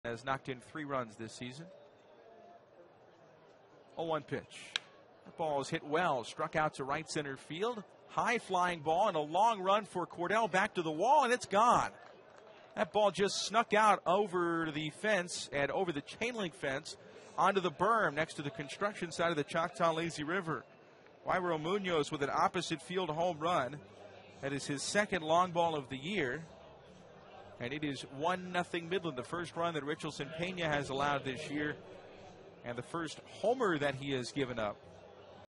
Midland's Yairo Munoz hits homer
5/9/16: Midland shortstop Yairo Munoz leads off the second inning with a solo home run